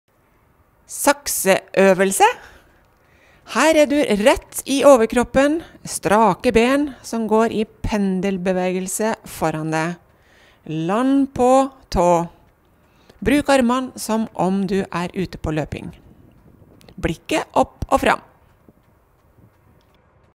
Treningstips: Sakseøvelse - Løpeglede
HVORFOR: For å styrke stabiliteten i hofta.

HVORDAN: Rett i overkroppen, strake ben som går i pendelbevegelser foran deg. Land på tå. Bruk armene som om du løper.